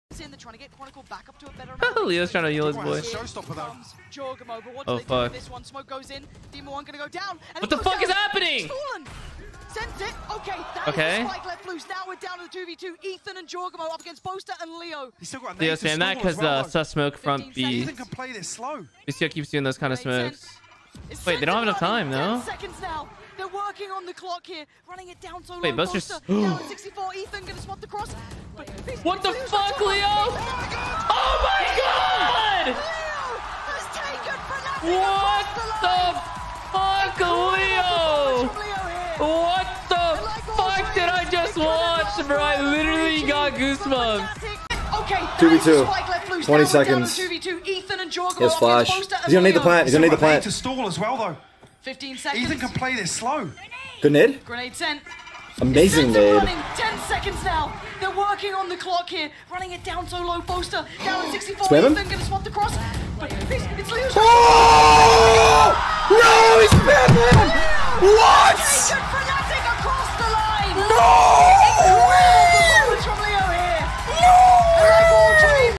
SEN Tarik & ShahZaM react to FNATIC LAST SECOND WIN against EG
FNATIC 2-1 EG 13-11 last map COMING DOWN TO THE LAST SECOND